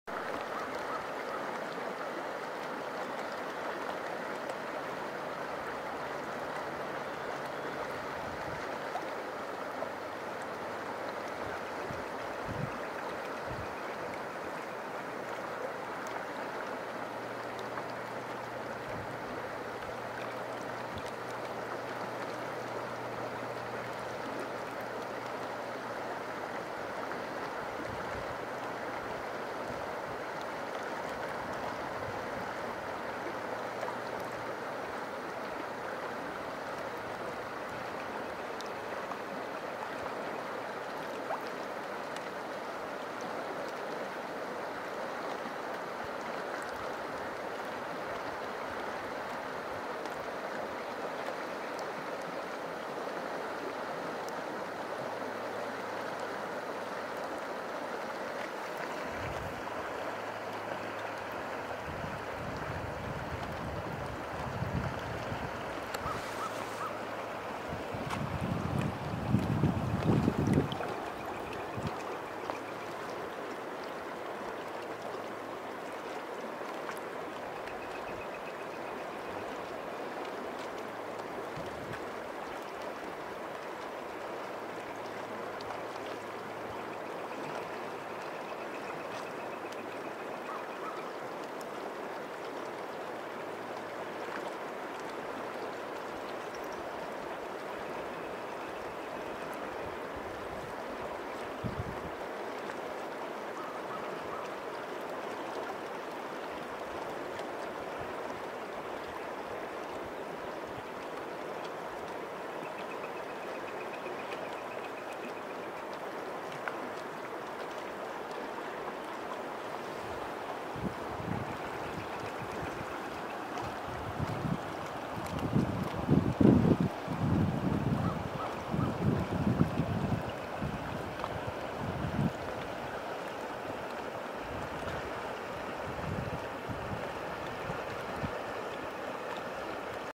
A Quiet Morning On The River
Taking a break on the bank of the river to enjoy some spring serenity.